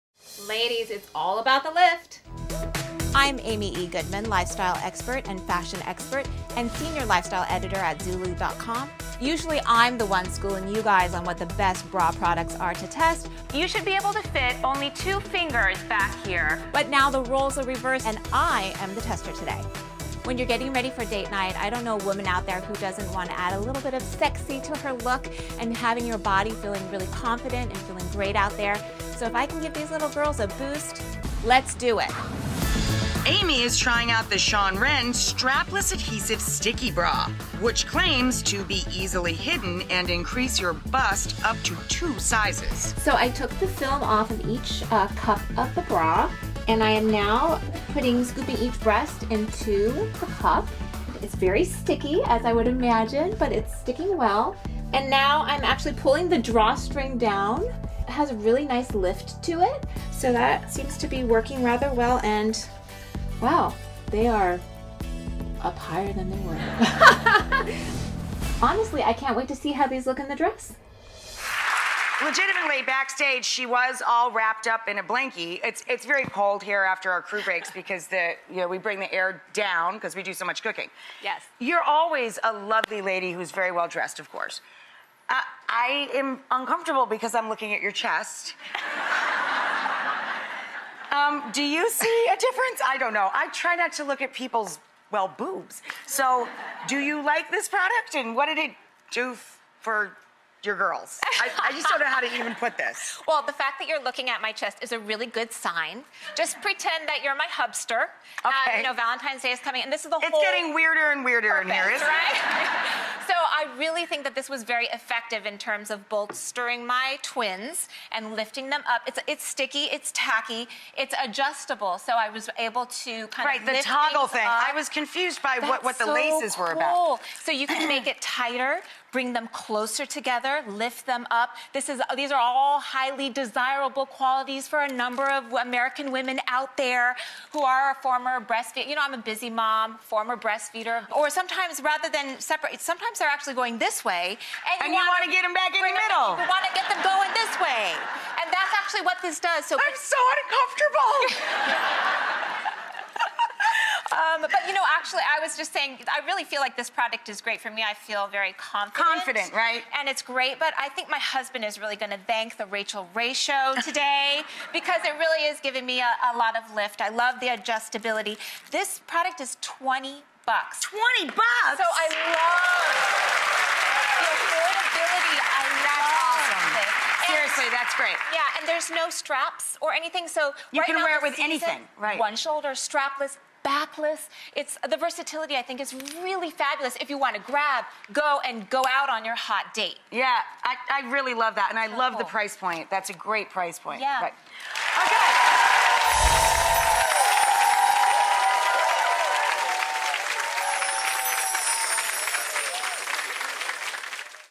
This Sticky Strapless Bra Claims to Boost You 2 Cup Sizes! We Put It to the Test
From an A to a C? Let's see!